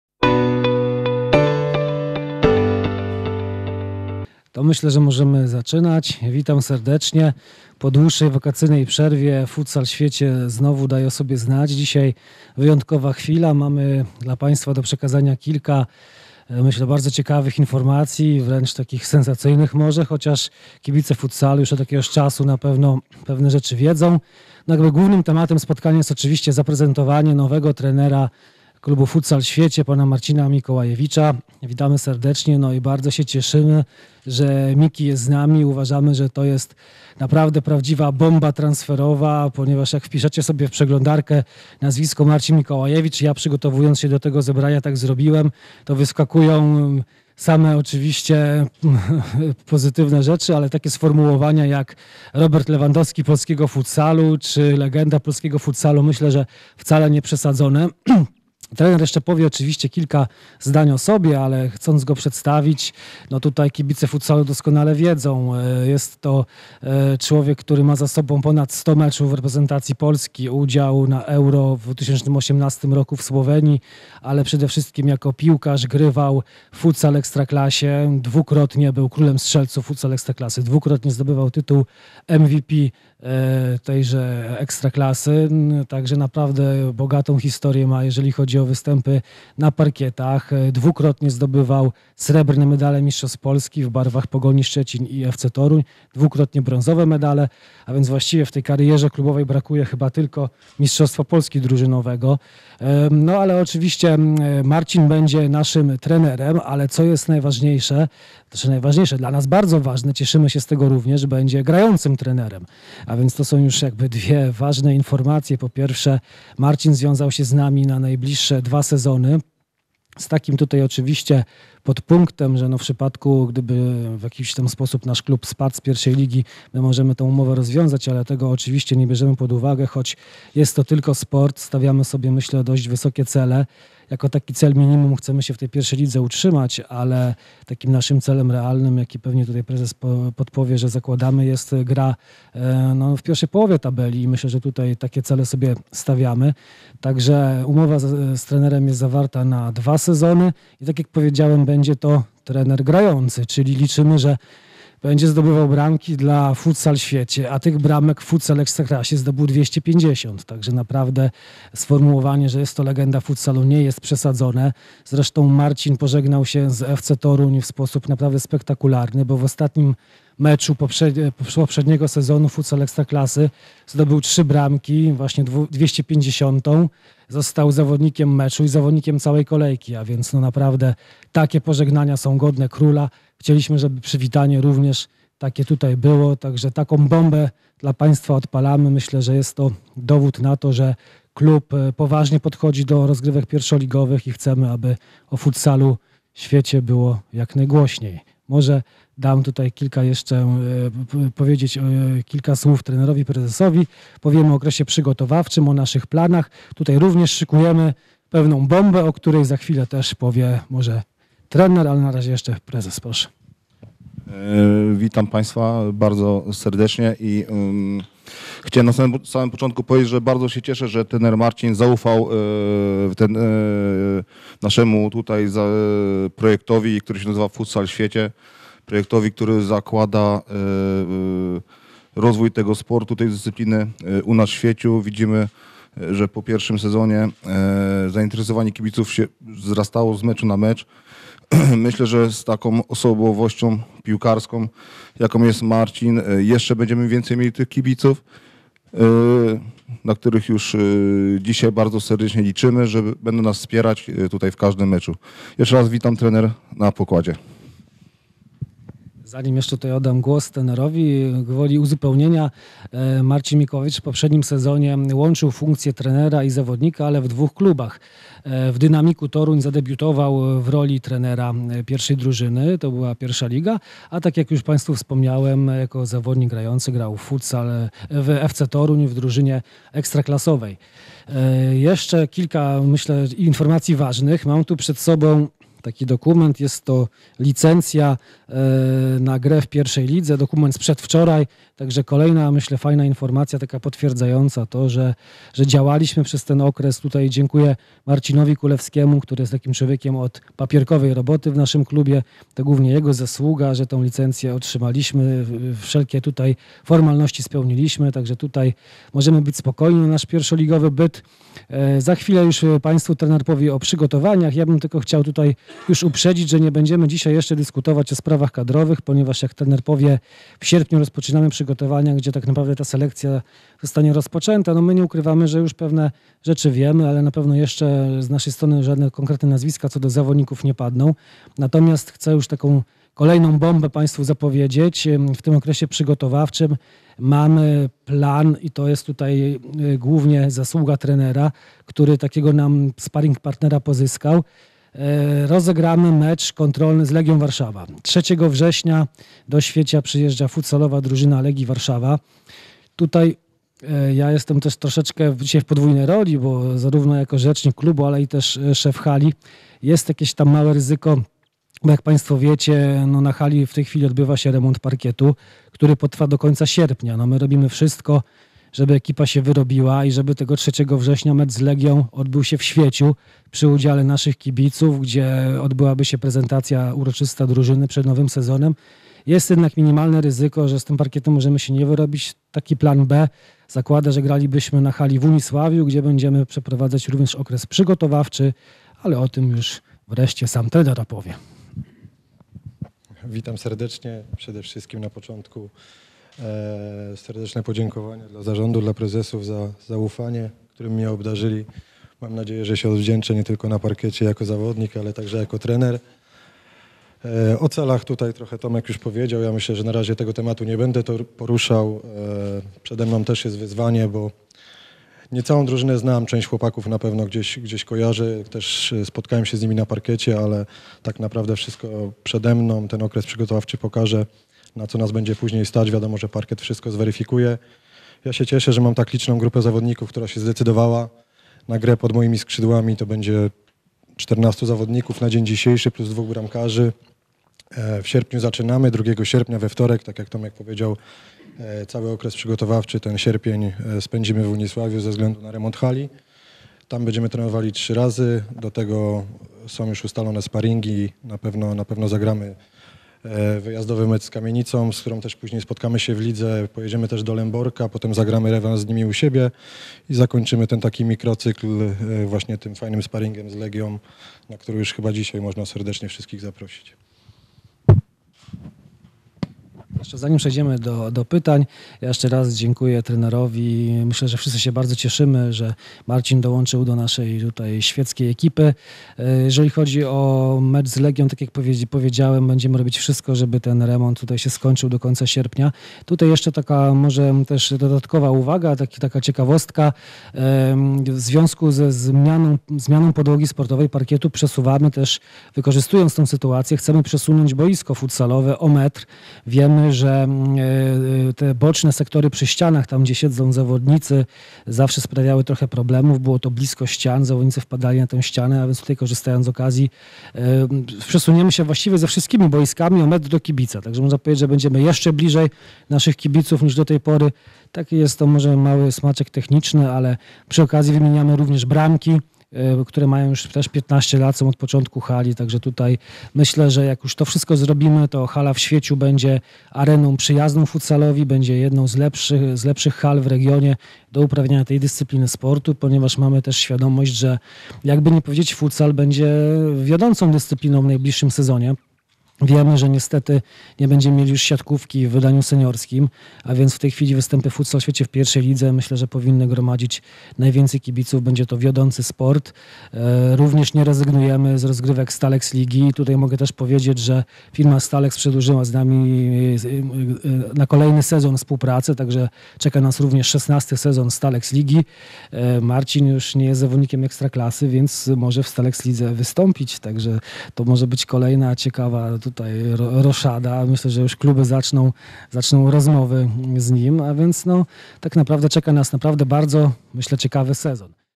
Konferencja klubu Futsal Świecie
Pierwsza konferencja prasowa klubu Futsal Świecie przed sezonem 2022/23